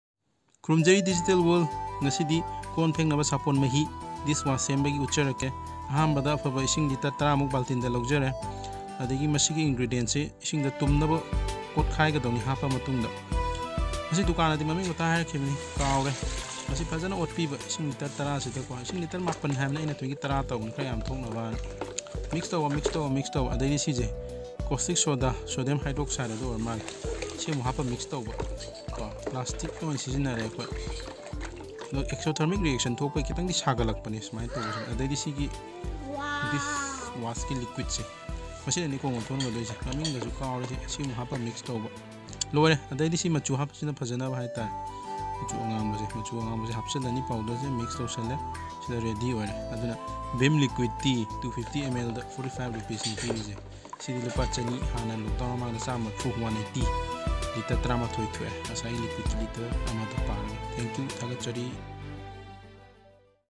How to make liquid dish wash
Making of liquid dish wash from ready made ingredients from shop